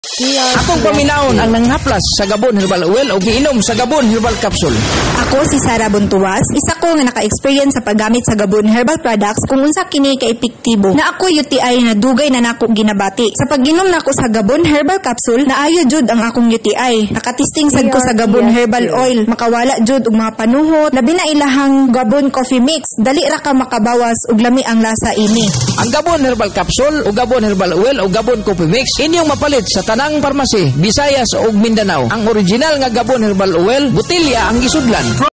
Gabon Herbal Oil and Capsule Radio Ad 2022-2023 35s (Testimonial Version 2, In Cebuano)
Ito po ang ikalawang testimonial Radio Ad ng Gabon Herbal Oil at Gabon Herbal Capsule na mabibili sa lahat ng botika sa Visayas at Mindanao. Based in Cebuano Language.

DRPS Two 5103: Gabon Herbal Oil and Capsule Radio Ad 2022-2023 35s (Testimonial Version 2, In Cebuano)